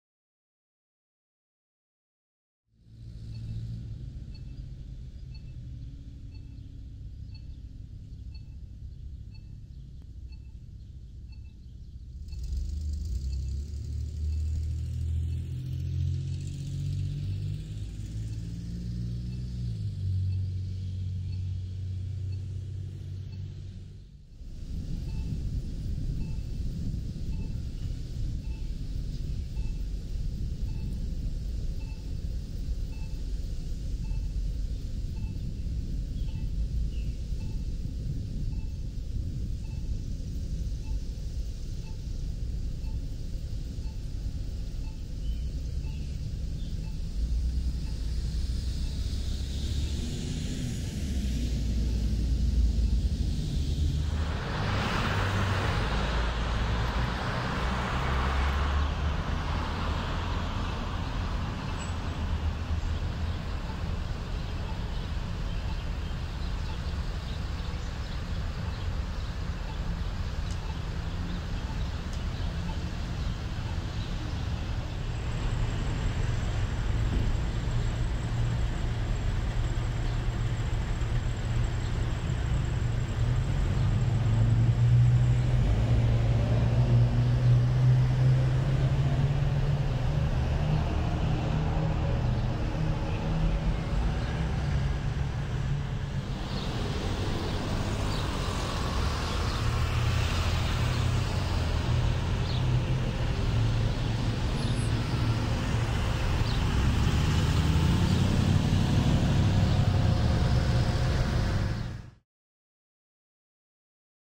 Berea College Square, Berea, Kentucky - Summer 2022
a lazy sunny Friday afternoon at Berea College Square in Berea, Kentucky Summer 2022
(sorry about the crosswalk "Beeps", I tried to filter 'em out a bit)

Shot with BlackMagic Cinema Camera 4K

Rokinon 12mm lens
Pentax 28mm lens